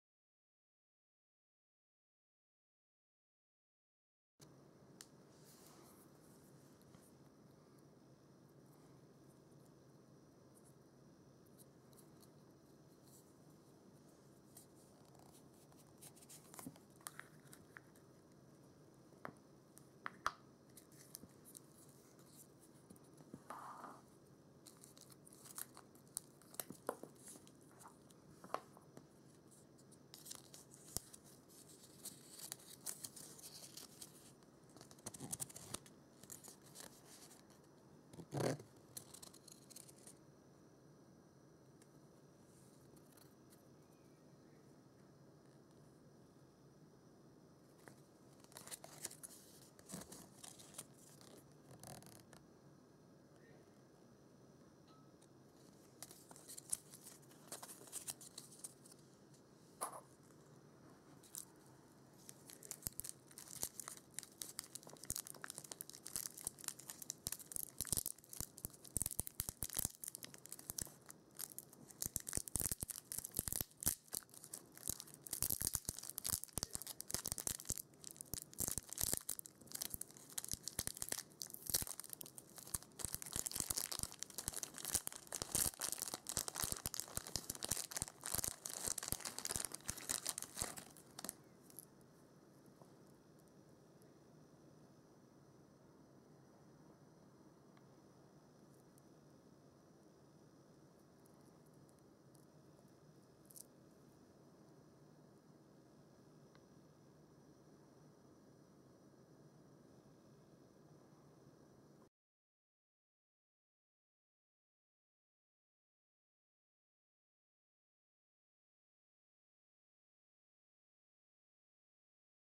ガシャポン キョウリュウジャースイング キョウリュウグリーン
『キョウリュウジャースイング』シリーズ『キョウリュウグリーン』の開封動画になります。